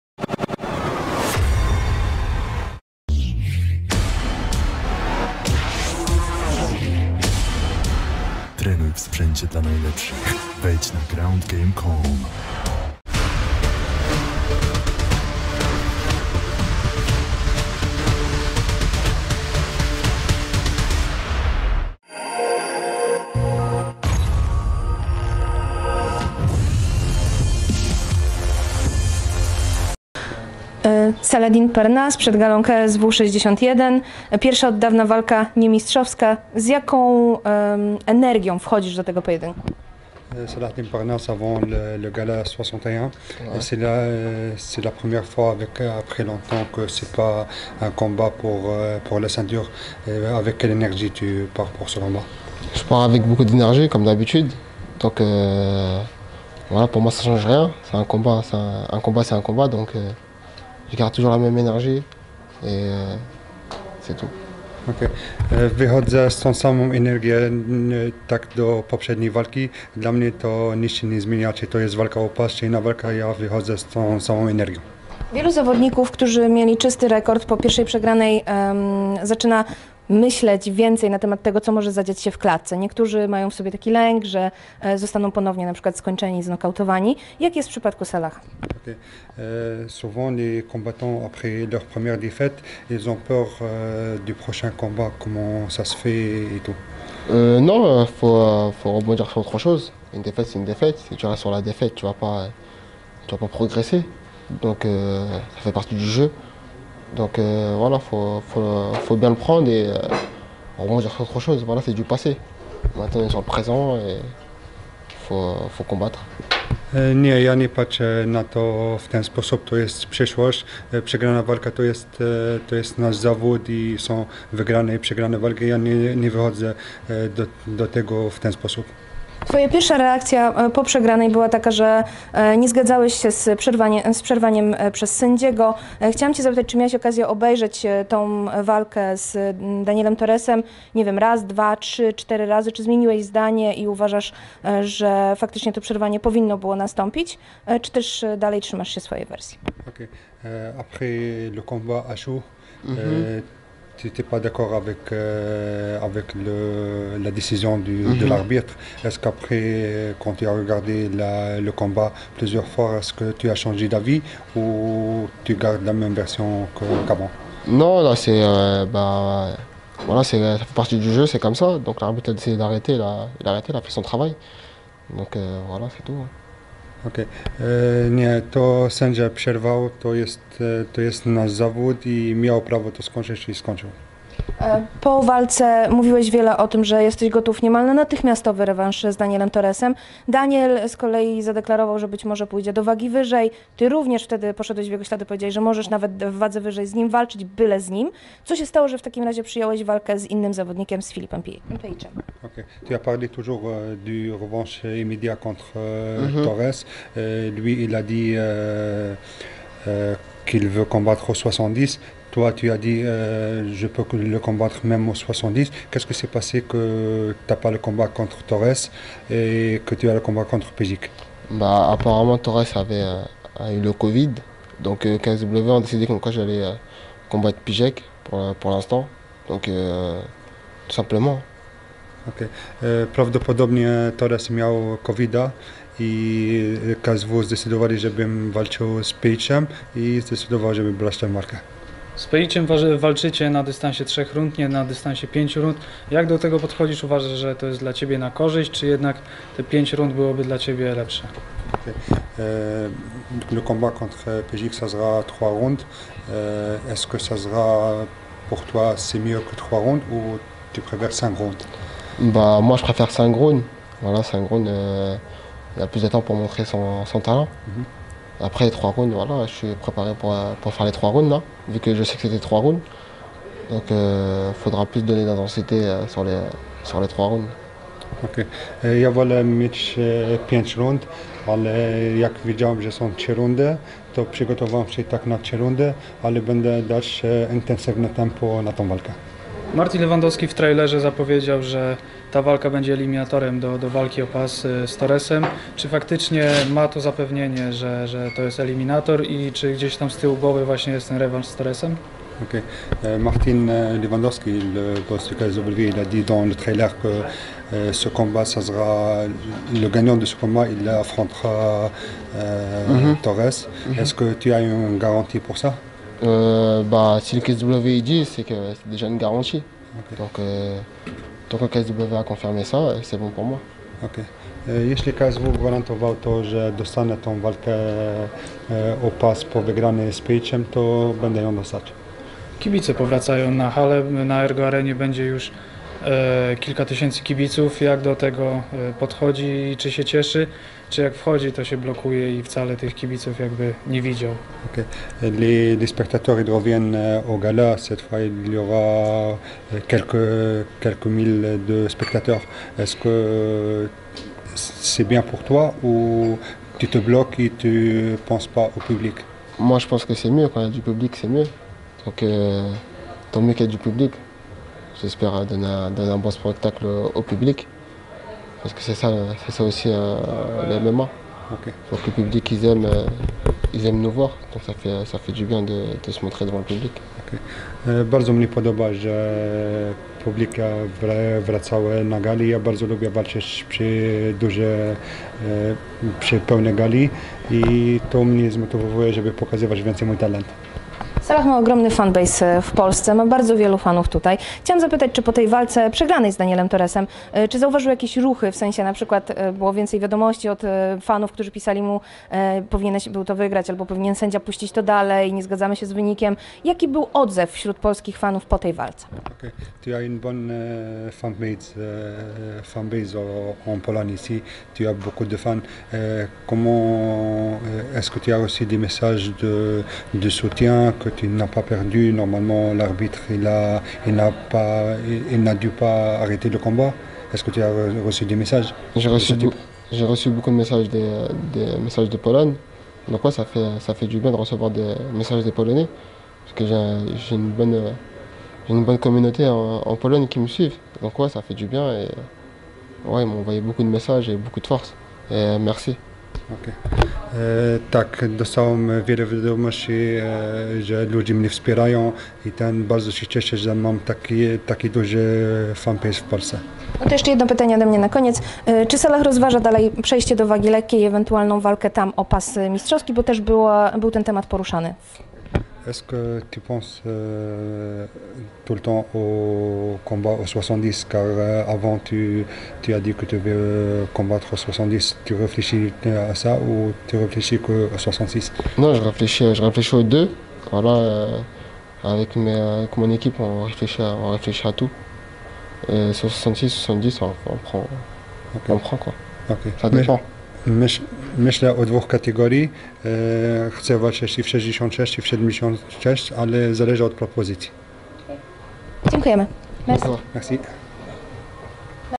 [PL] Salahdine Parnasse o przerwaniu w walce z Torresem i planach na starty w dwóch dywizjach
Kup dostęp PPV do KSW 61 w niższej cenie do końca maja ⤵

Nasi partnerzy:
👉 GROUND GAME: 10% rabatu z hasłem "ITC":